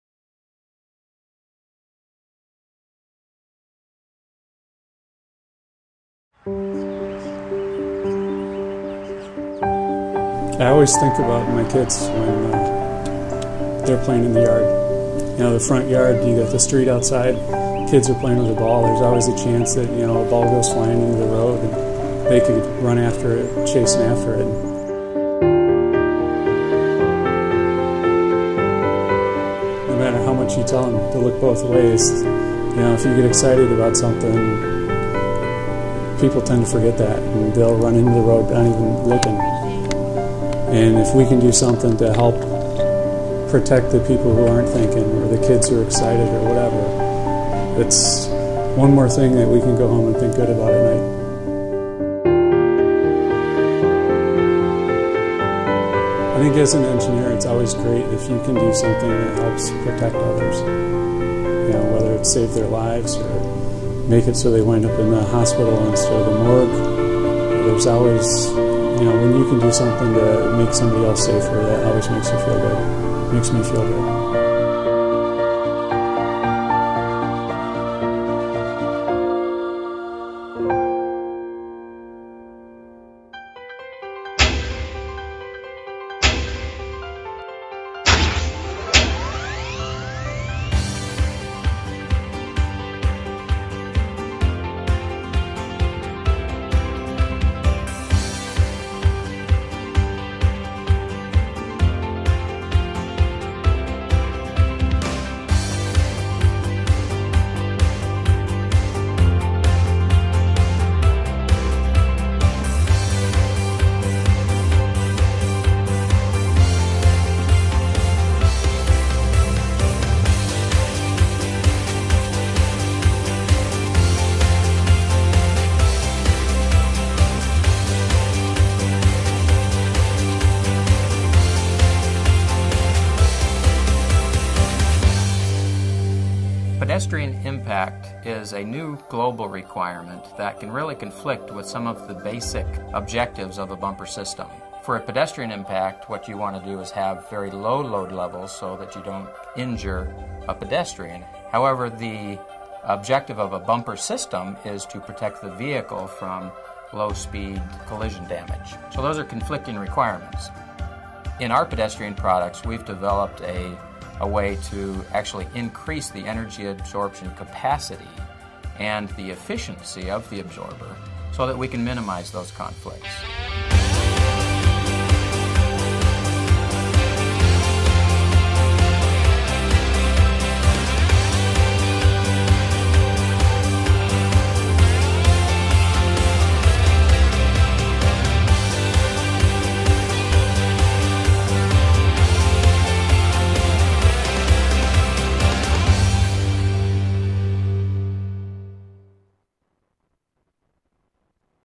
Shape Corp. is dedicated to Pedestrian Protection
Leaders in impact energy management, Shape Corp. is taking action to better engineer vehicle components to protect pedestrians in event of a motor vehicle to pedestrian impact. Shape-NetShape is a global leader in Pedestrian Protection.